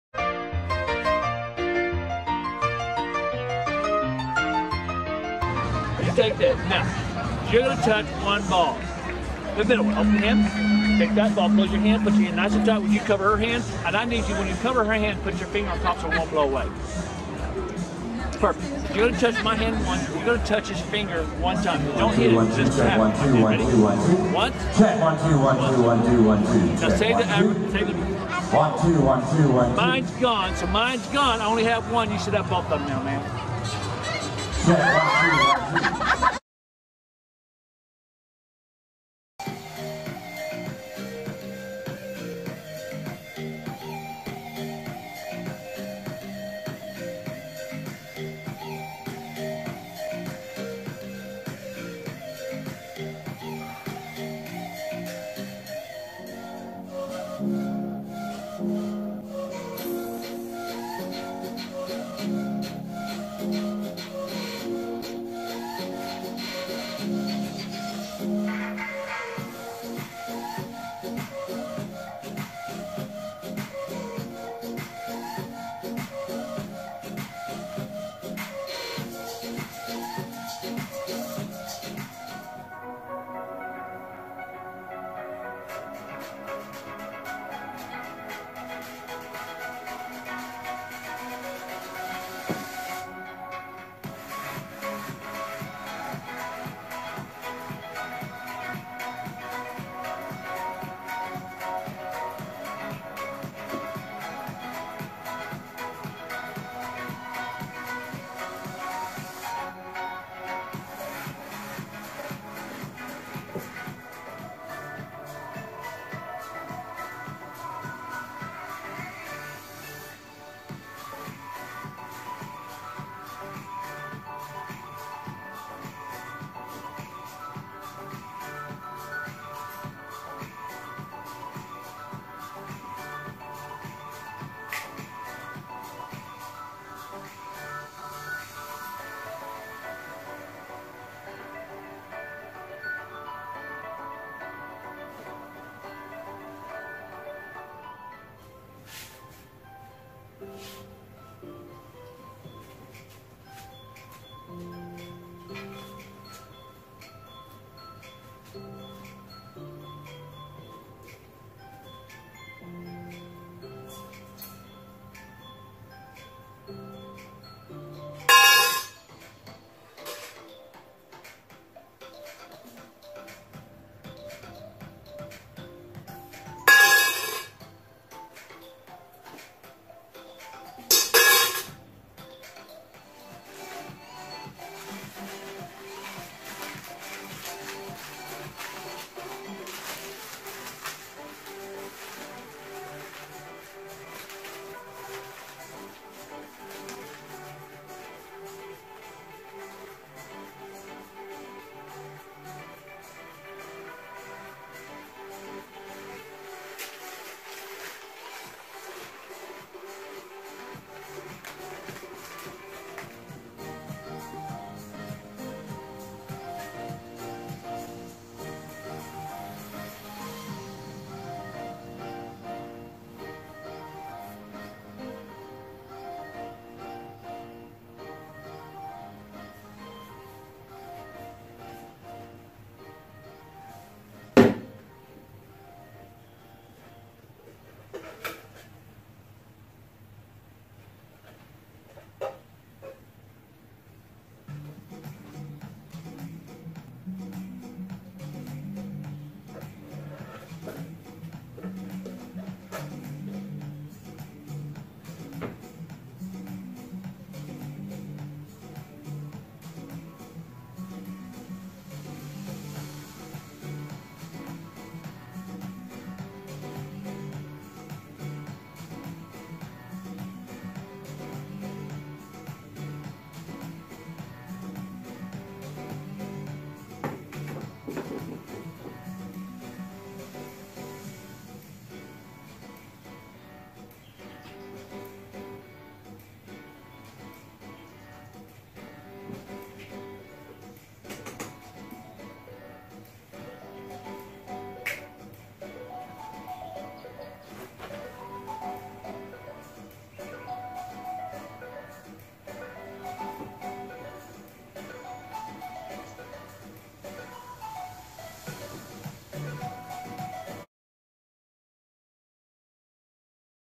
Family Fun Magic
Magician Marty Westerman performs close-up sleight-of-hand strolling magic as well as small stage comedy shows with audience participations. A fun experience for everyone!